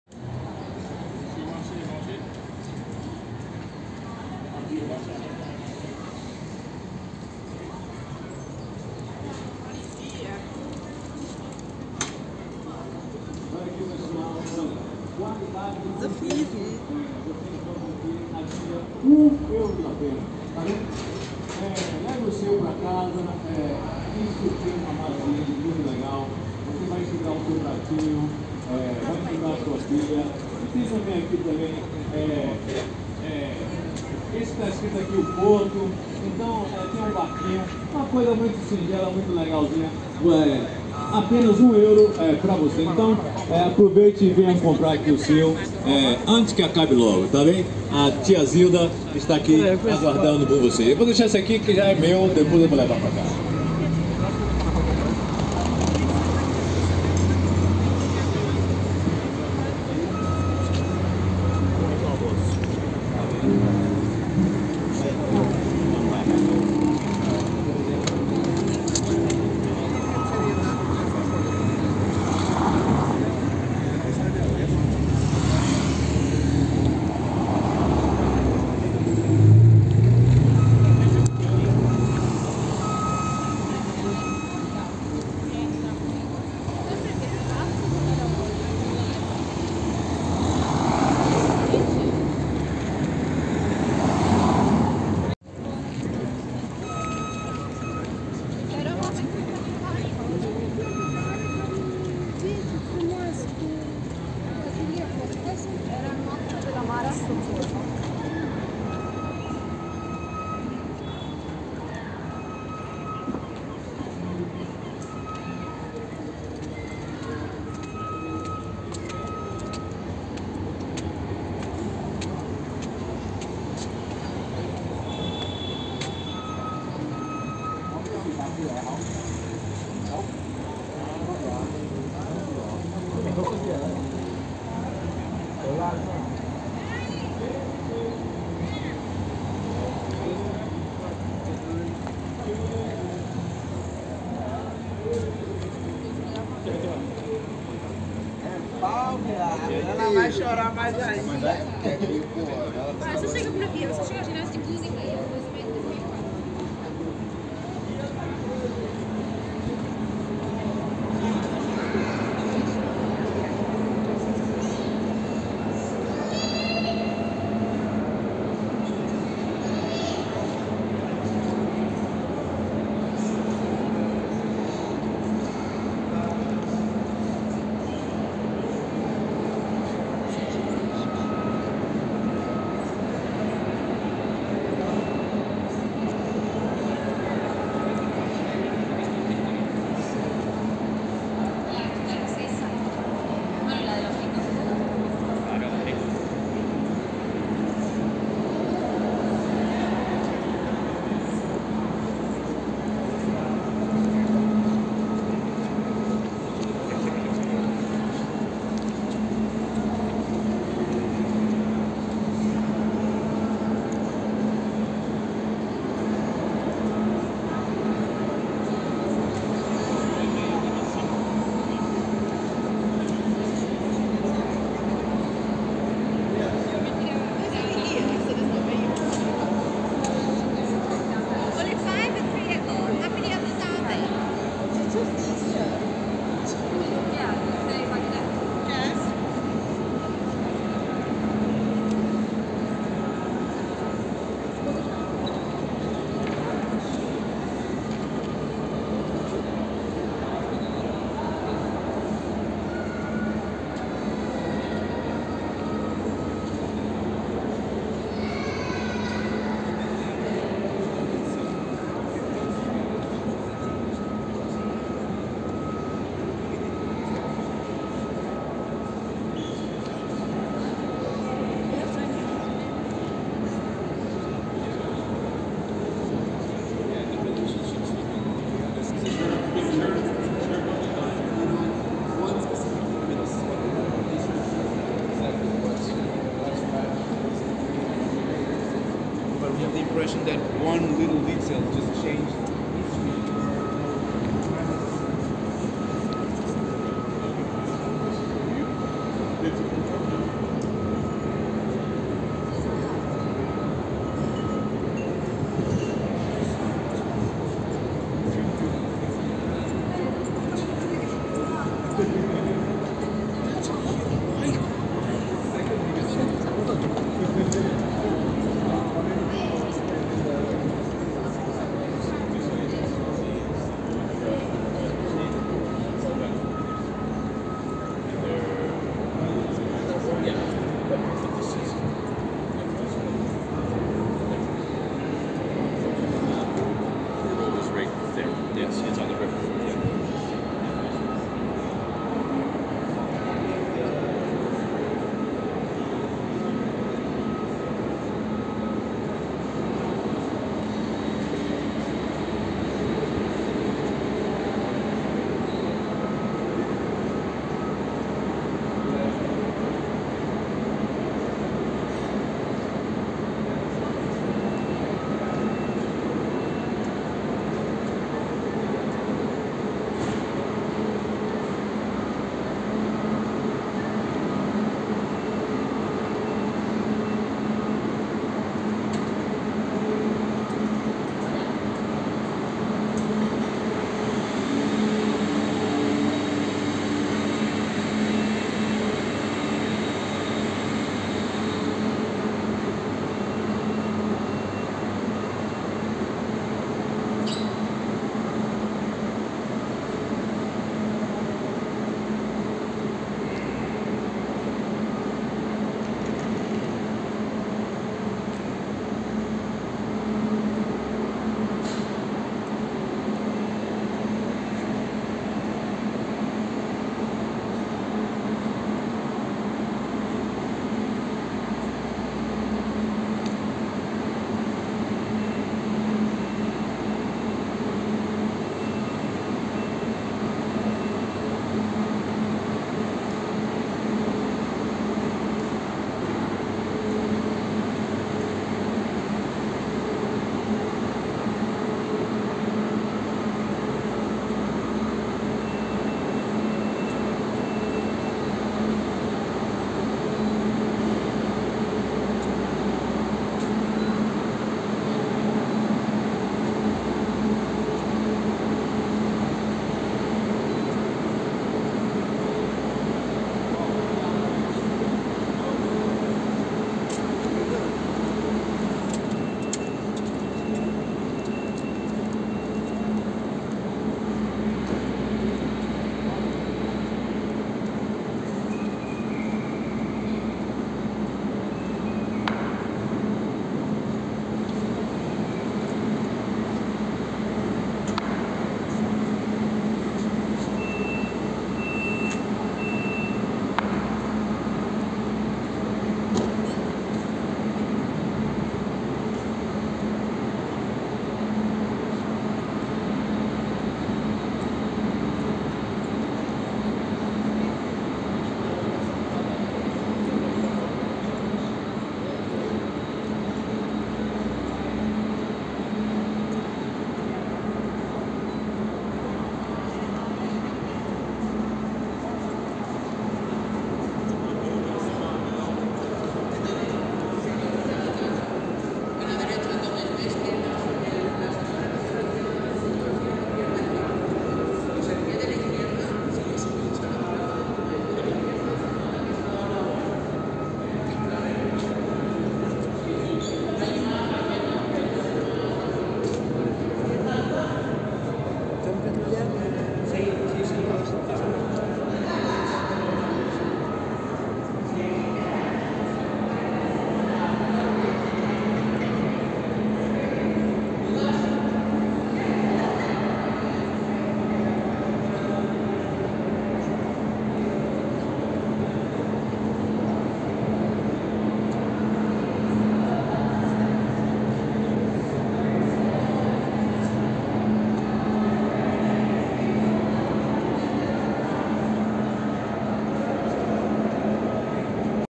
Walking Tour in São Bento Station, Porto🇵🇹
In this Vlog:

"Welcome to Sao Bento Station in Porto! In this video, I'll take you on a tour of one of the most beautiful and historic train stations in the world. Filmed on March 2023, this video showcases the stunning architecture and intricate tile work that makes Sao Bento Station such a special place. Learn about the history of the station, its significance to the people of Porto, and the stories behind the intricate tile murals that adorn the walls. Sit back, relax, and enjoy the sights and sounds of Sao Bento Station in this immersive video tour.